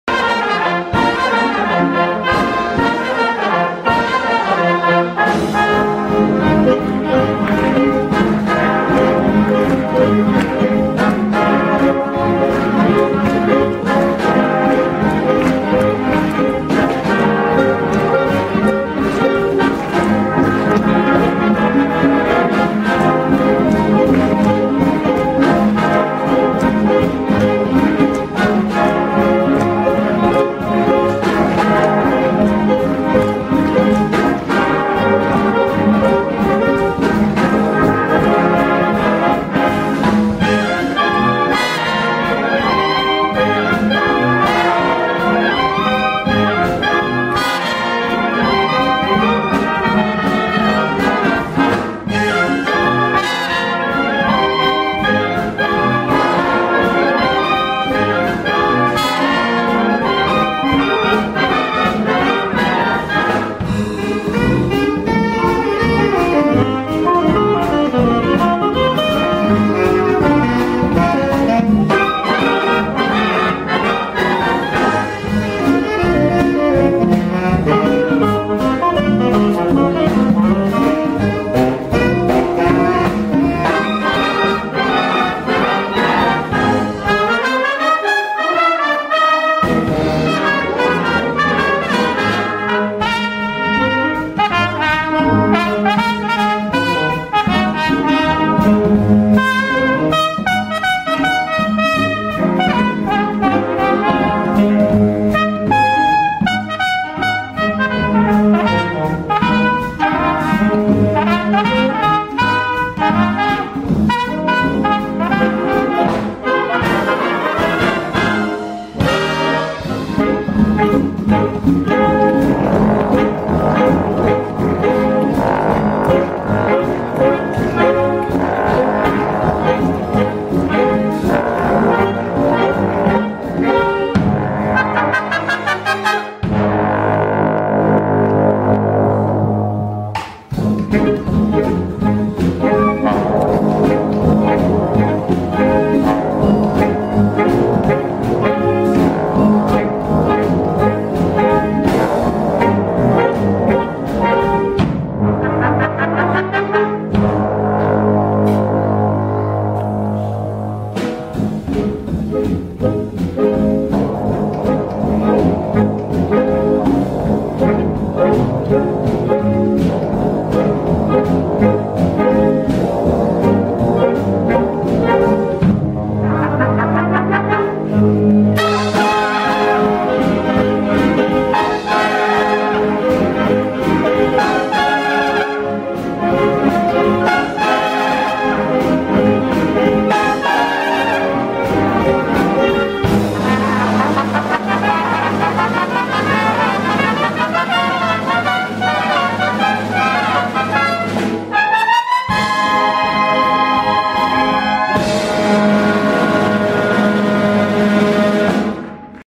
Concert de jazz avec la musique de Rixheim
La musique municipale de Rixheim a donné un beau concert de jazz le samedi 3 mai 2014 à l'Espace Rhénan de Kembs au profit de l'Association La Vie en Marche.